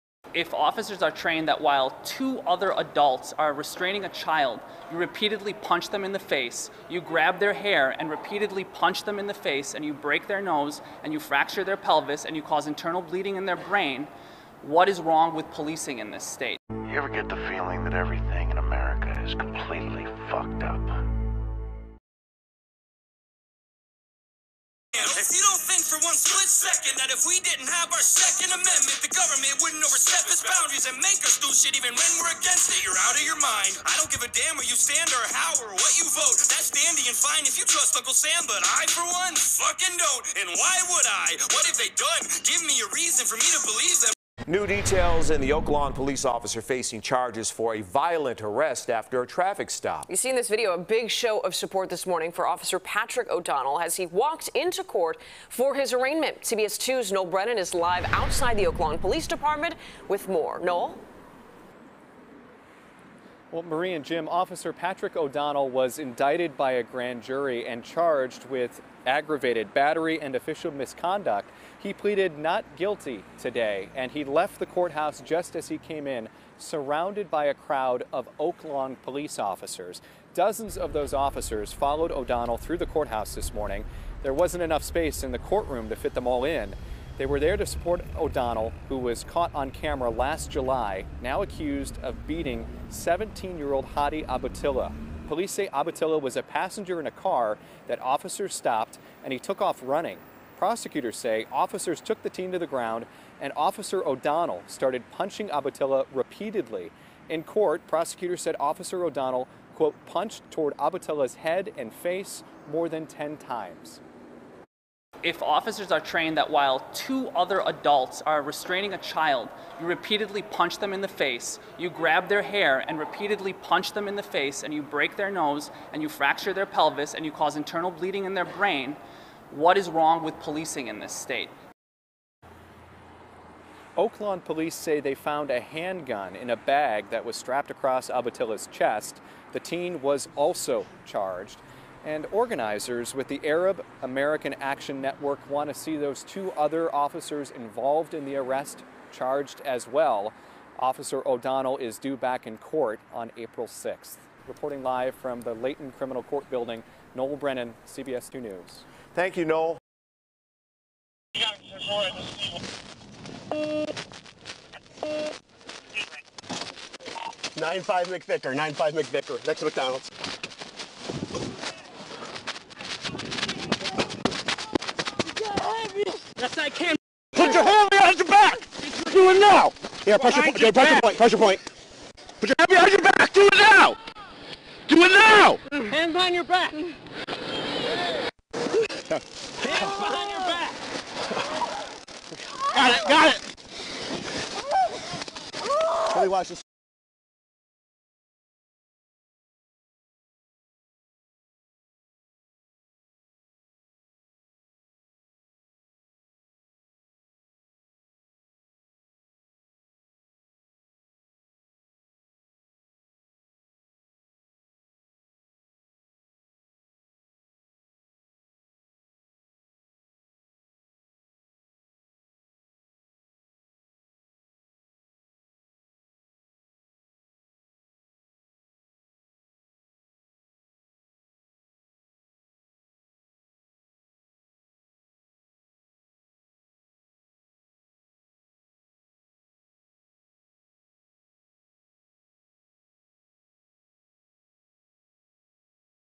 Oak Lawn Illinois ~ Officer Patrick O'Donnell charged battery & official misconduct
Teen runs from a traffic stop and is chased down, beaten and tased.